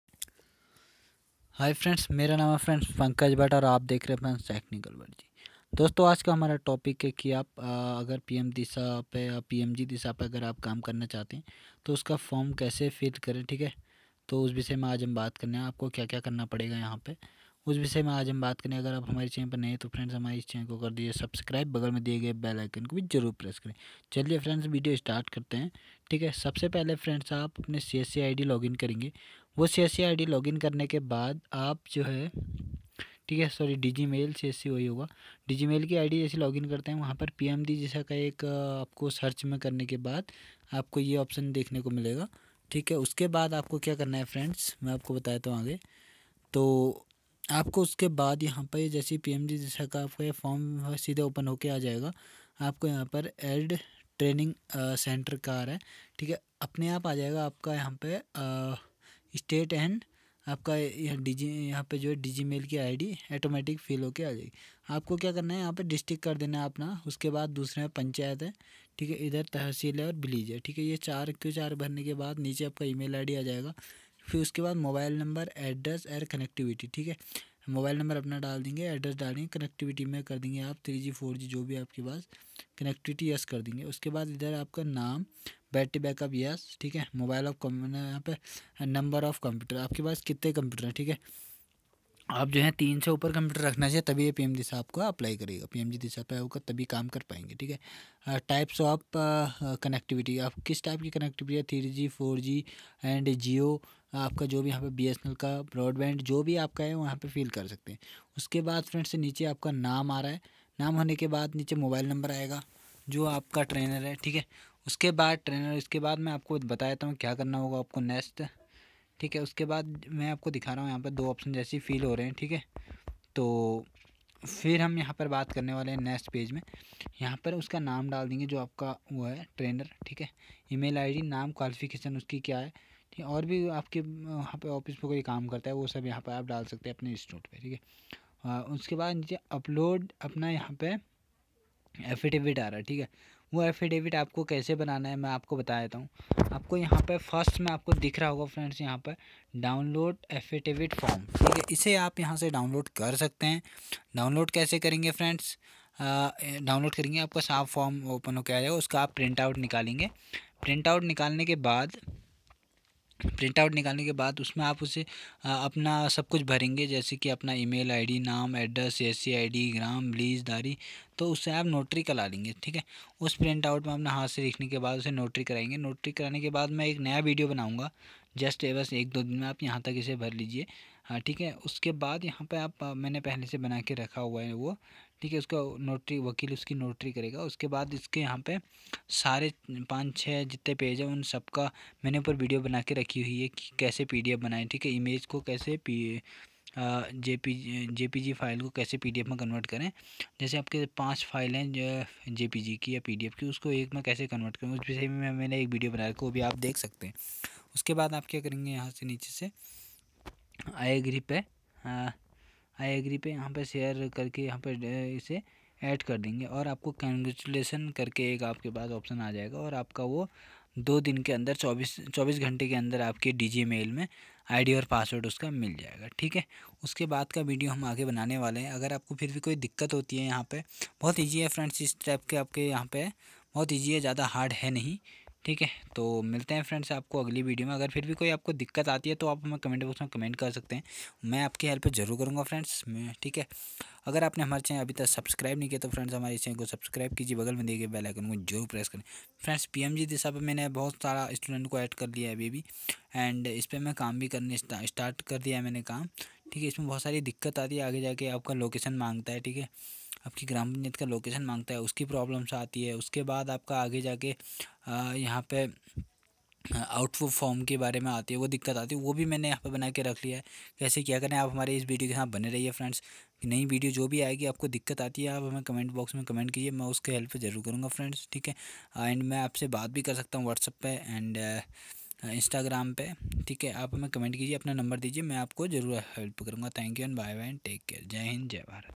Pmgdisha Registration full process 2019 | PMGDISHA पर रजिस्टर कैसे करें हिंदी में सीखो 2019
New VLE Pmgdisha Registration prossing 2019 | PMGDISHA कैसे रजिस्टर करें हिंदी में सीखो 2019
technical bhattji
new vle registration 2019
 hii friends my name pankaj bhatt